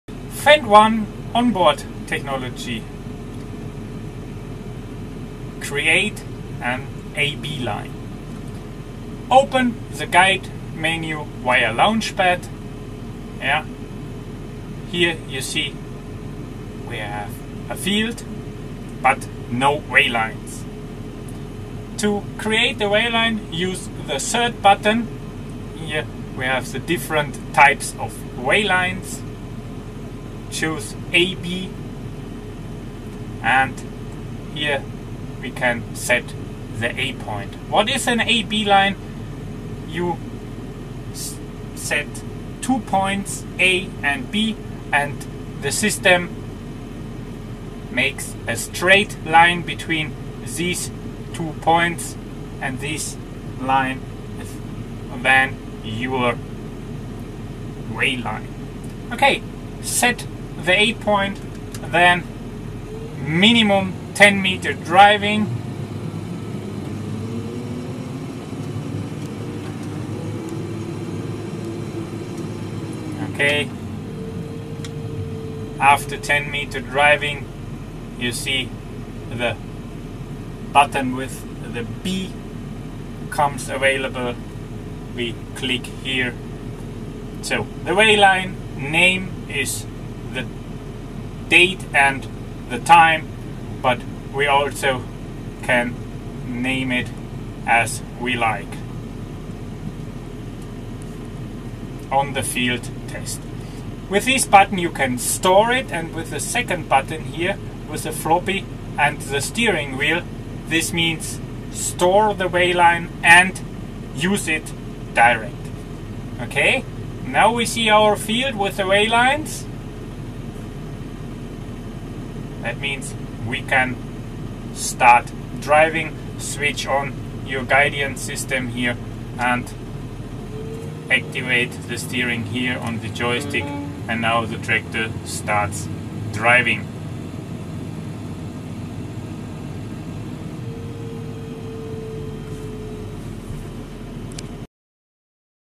How-to | FendtONE onboard | Part 24 | Create AB line | Fendt
24 Creating an AB Line- This video explains how to create an AB line for tracking and what is meant by an AB line.